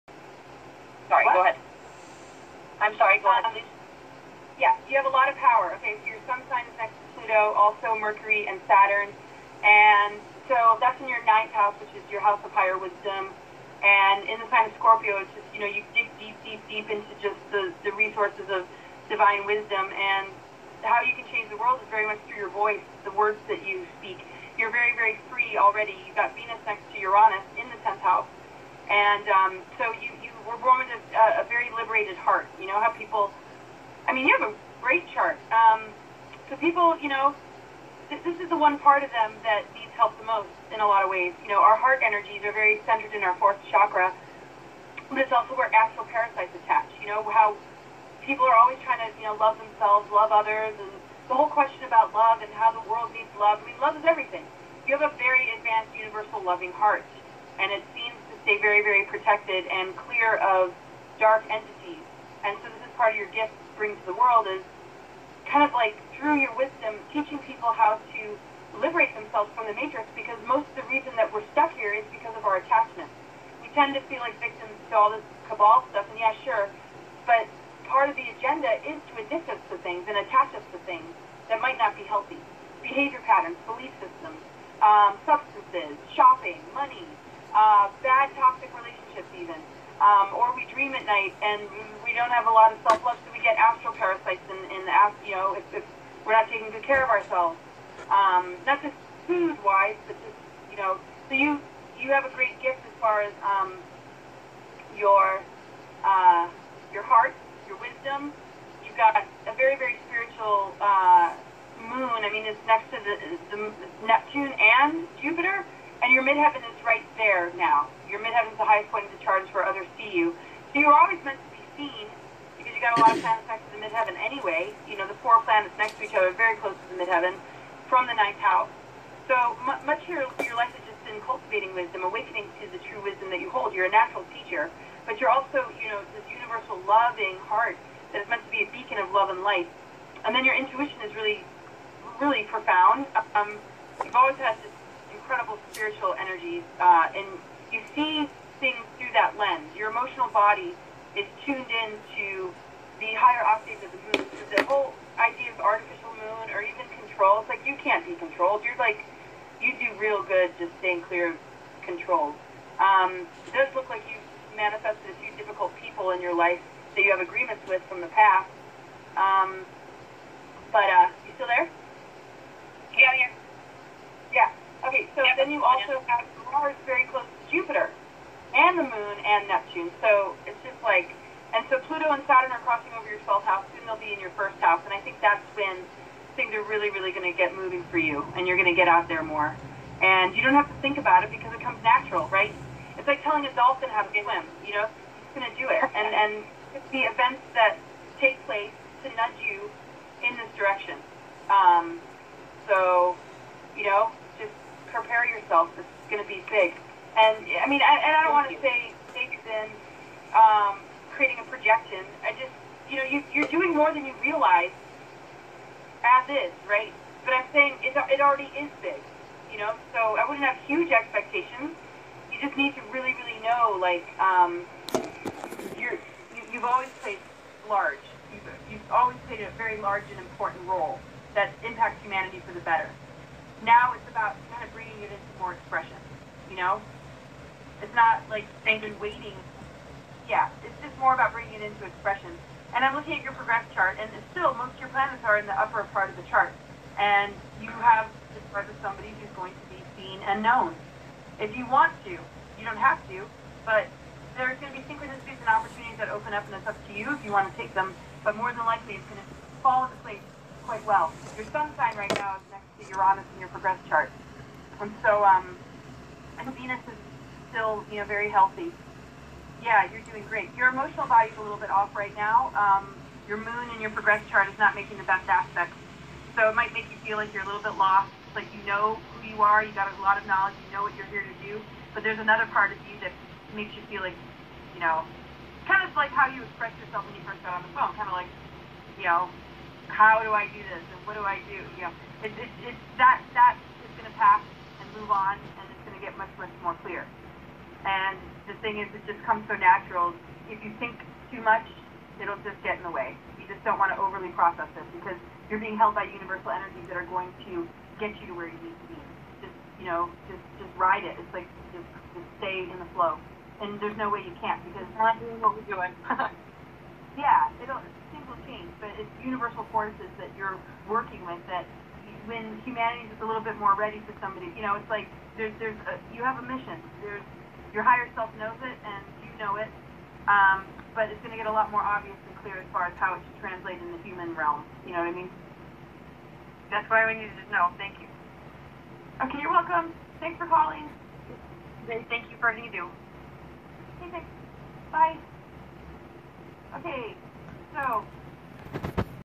Brief Analysis Of My Natal Chart By Laura Eisenhower
Laura Eisenhower founder of cosmicgaia.org  is an astrologer and great grand daughter of the 34th president of the United States.
Please support her on her website. She does stuff like the call in astrology shows all the time. She is helping to save the world every single day and we should all be thankful for the work she does.

But enjoy, this is my chart she's reading.